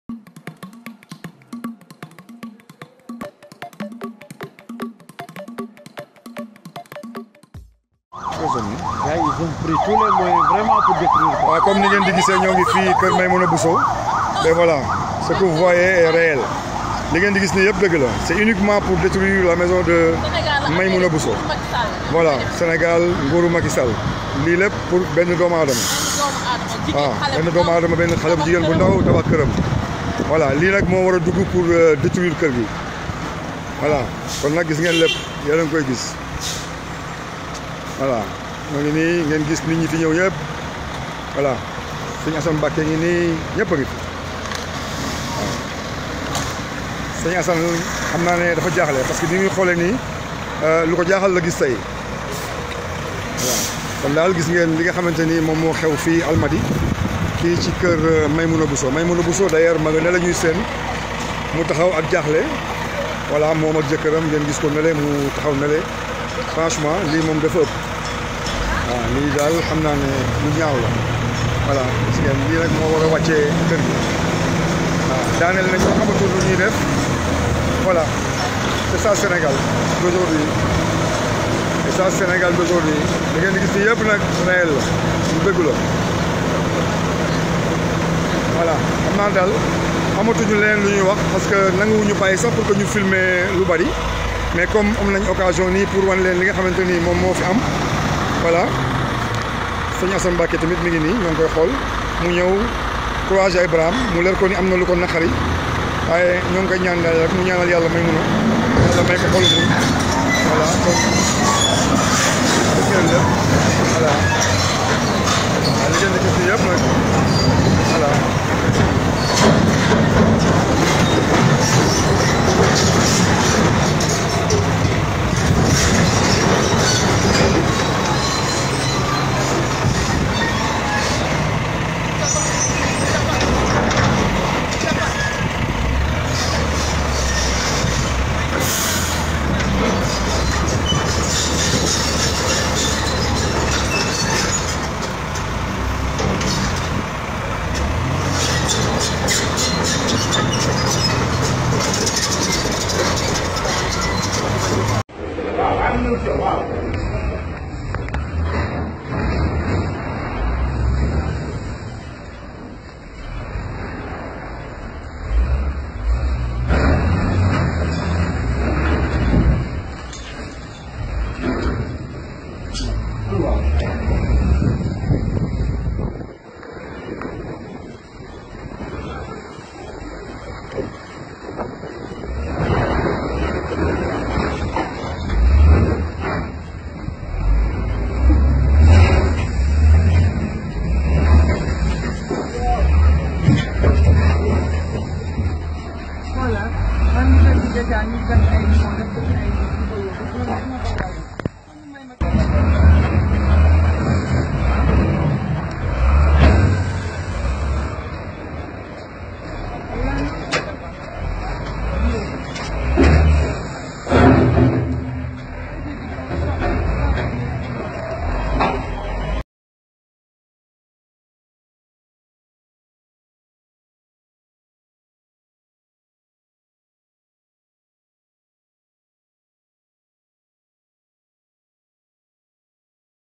Urgent – Démolition en cours de la maison de l’opposante Maimouna Bousso
N'oubliez pas le pouce en l'air si vous avez apprécié, merci à tous :)